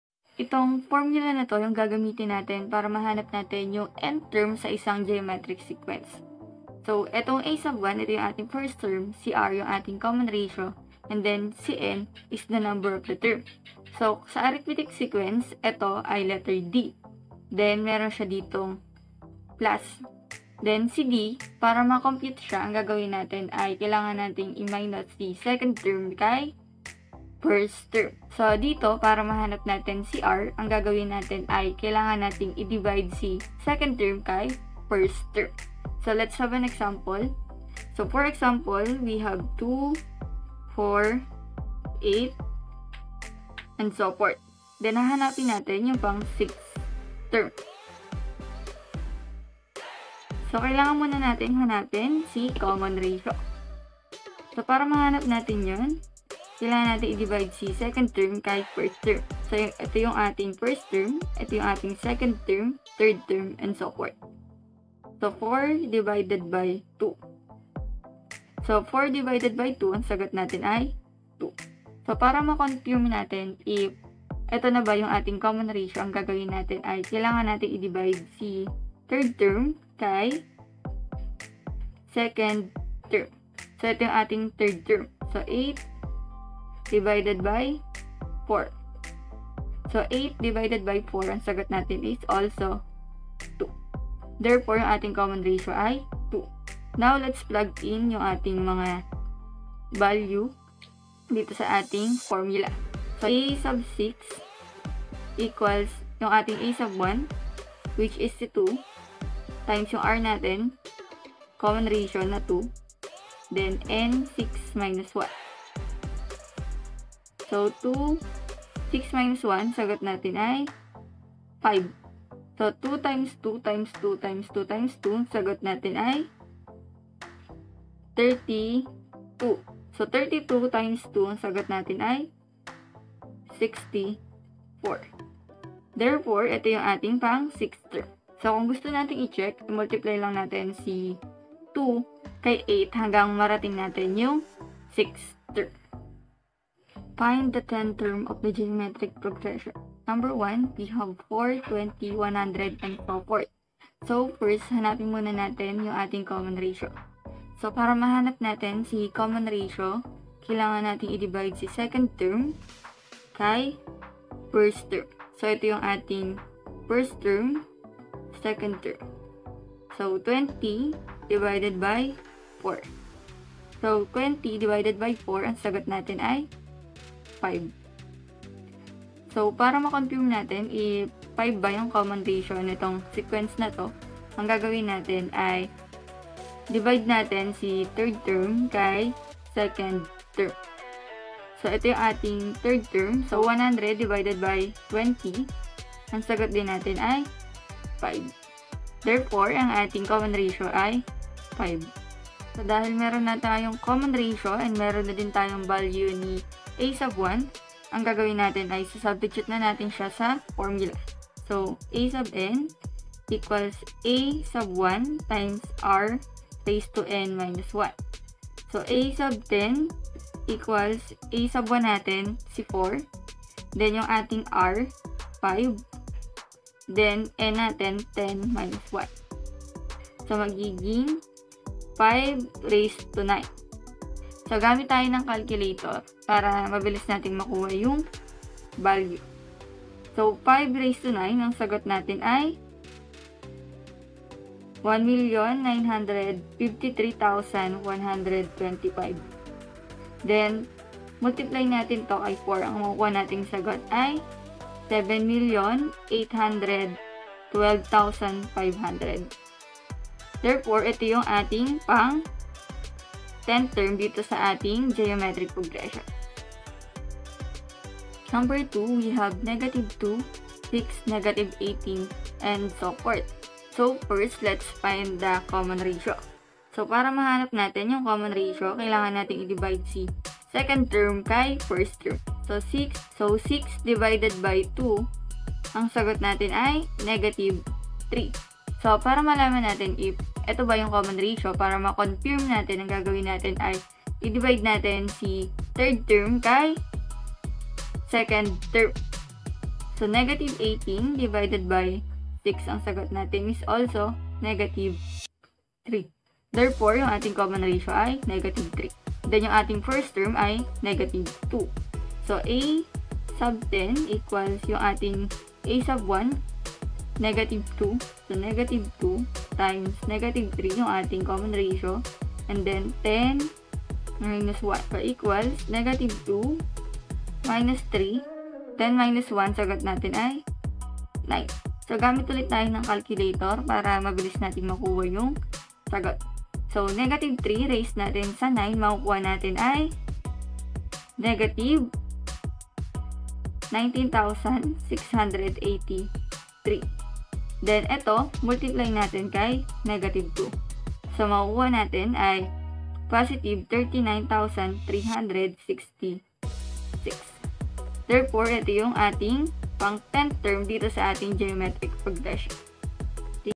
TAGALOG: MATH 10 - Find the 10th term of the GEOMETRIC PROGRESSION | GEOMETRIC SEQUENCE
Find the 10th term of the GEOMETRIC PROGRESSION | GEOMETRIC SEQUENCE